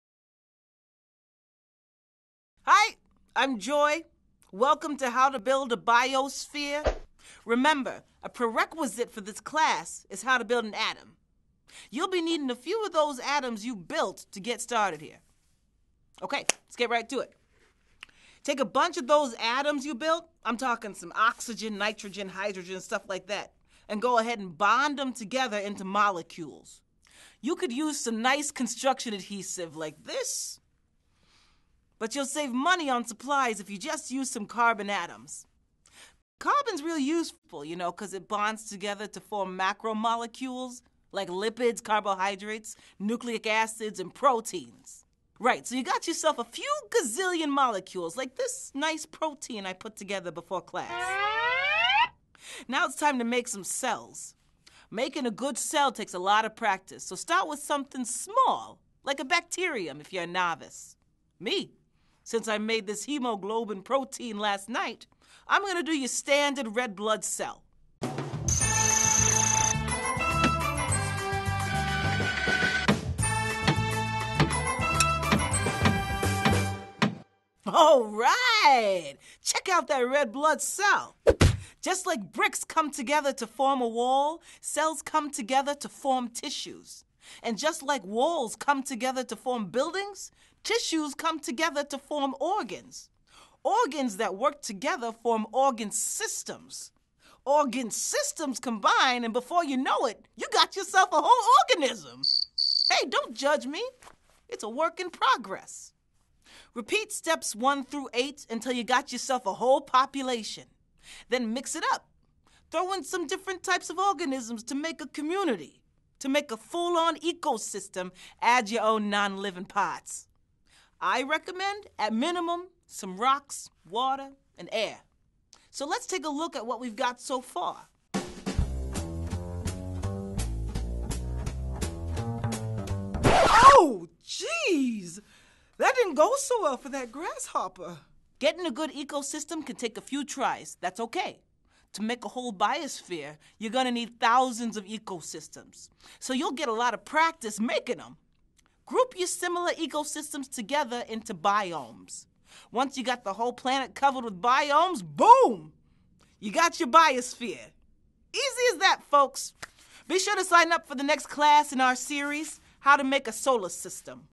From Atoms to Biosphere: Biology Tutorial
See the Review It videos from Apex Learning Tutorials in action! These high-impact videos engage students with an overview of objectives covered in the topic.

Relevant, real-world examples are included to help students connect what they are learning to their own life. These videos are great for visual learners who prefer to see concepts demonstrated. In this case, the From Atoms to Biosphere video shows students how this Biology concept works by showing them how it works.